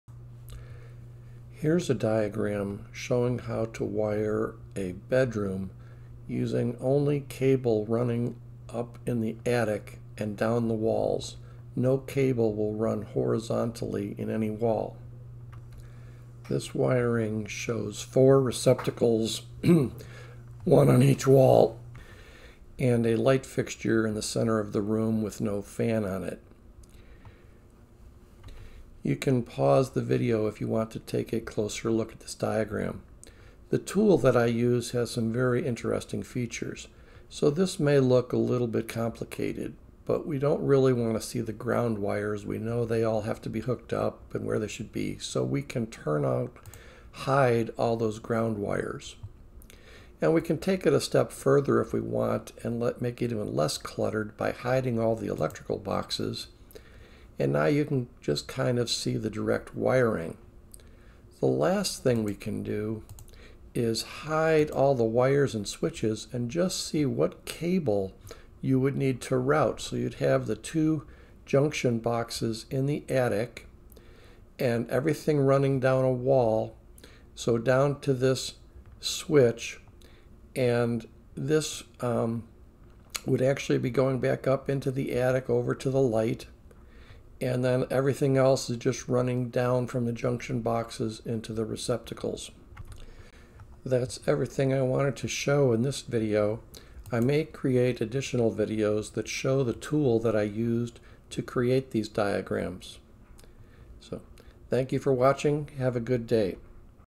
Wiring bedroom using attic junction boxes (Version 2)
Shows a diagram for wiring a bedroom with 4 receptacles and a ceiling light with wire originating in attic junction boxes.  Feel free to critique and/or point out any mistakes.
This video is an improvement from the original video.